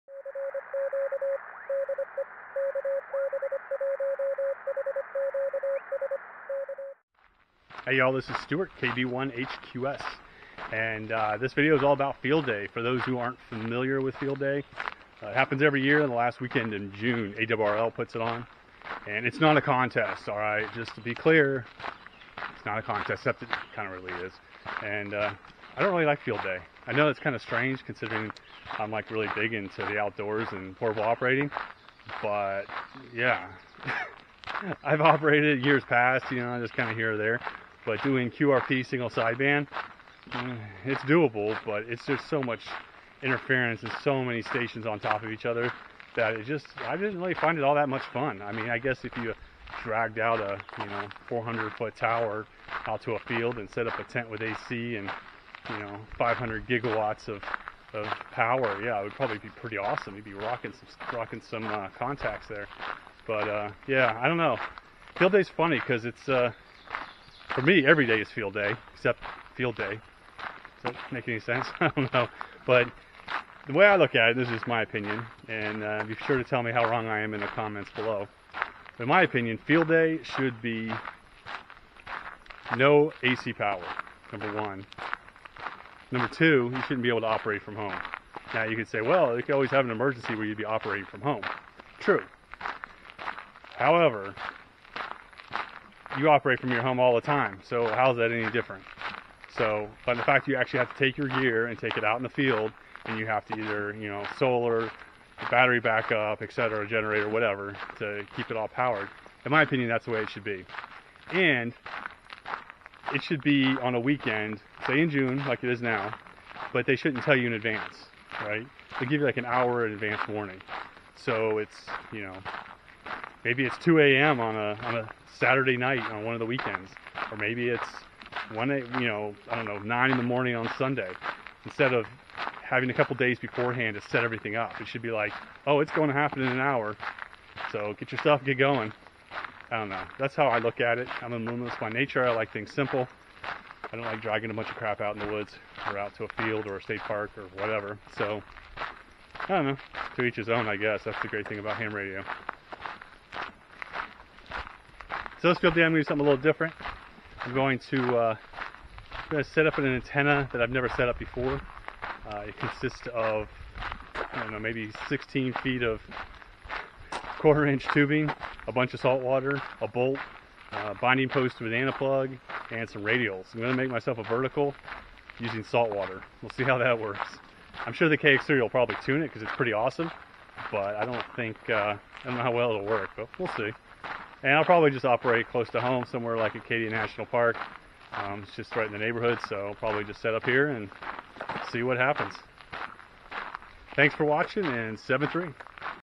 2020 Field Day & my Salt Water 20m Vertical Antenna
In this video I talk about my thoughts on Field Day and my plans for a saltwater vertical 20m antenna.
73,
Stuart KB1HQS.com

ARRL Book.